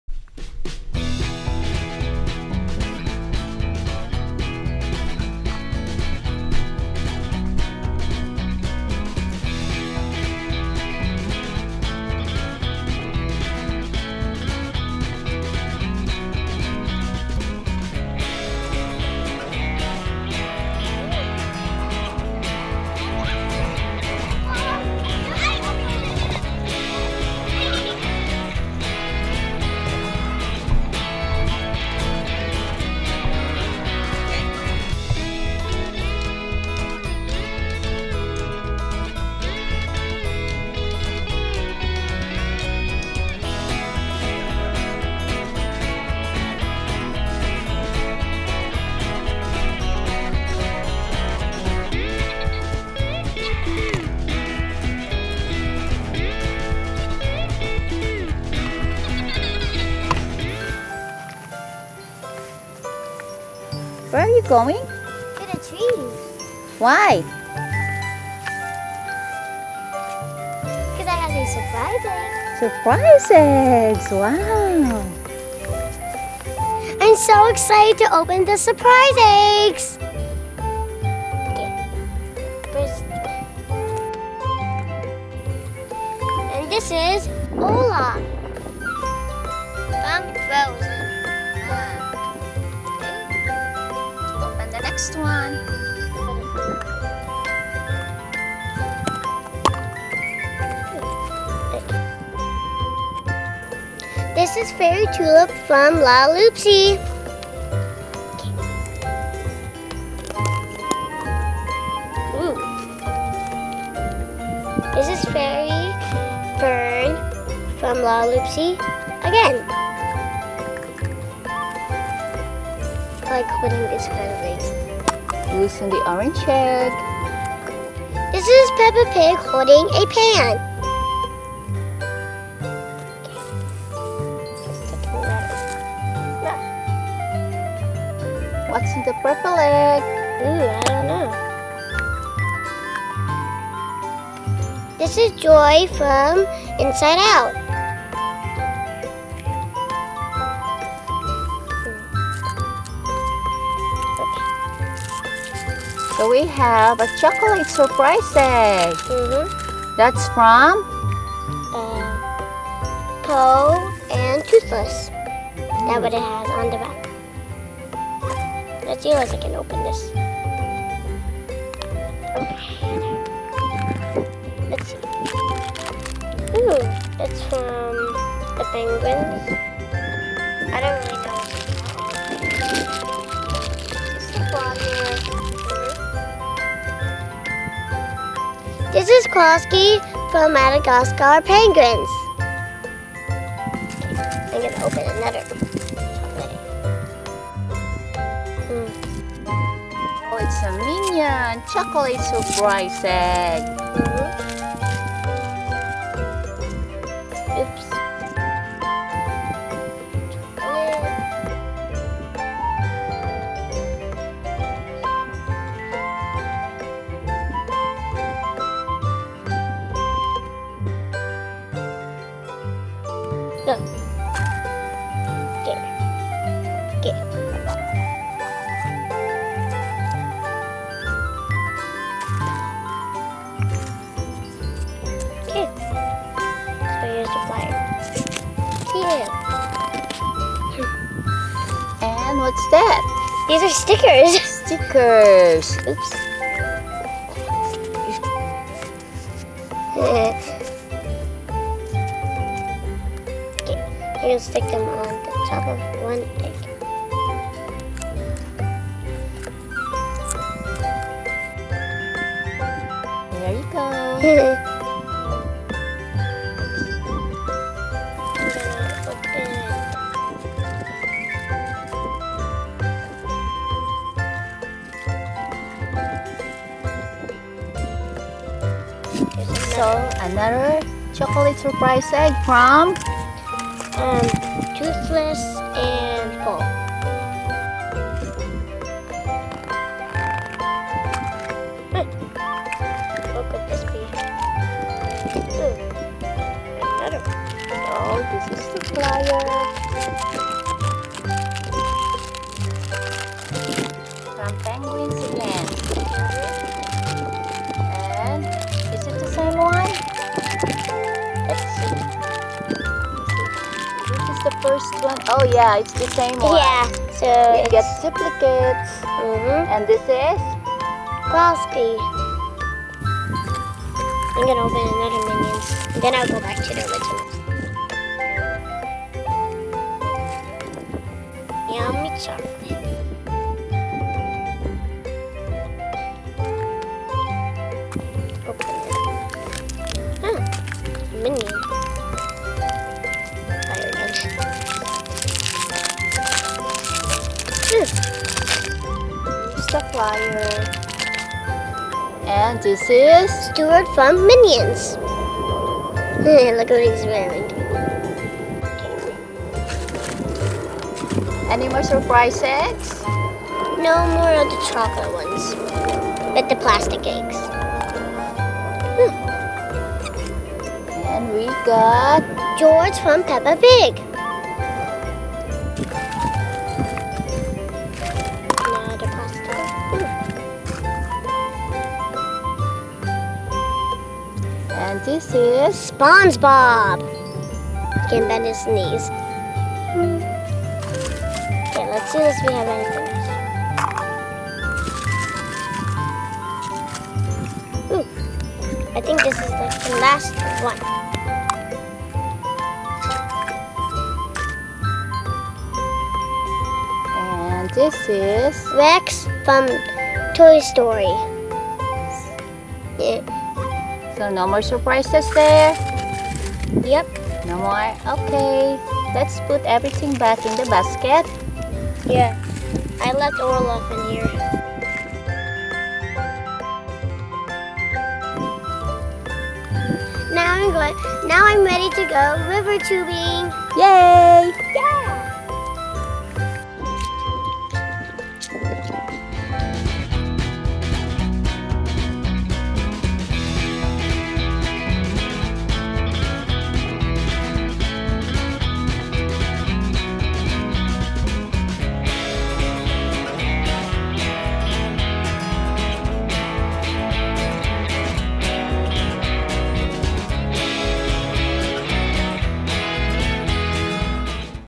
Fun River Adventure + Surprise Toys| Toys Academy
Ms K had an amazing fun time playtime at the river with her friends and family. She went river tubing. And she also opened 12 surprise eggs with Peppa Pig, Joy of Inside Out, Lalaloopsy fairies, Minions, Penguins, Spongebob, Olaf, Rex the dinosaur from Toy Story.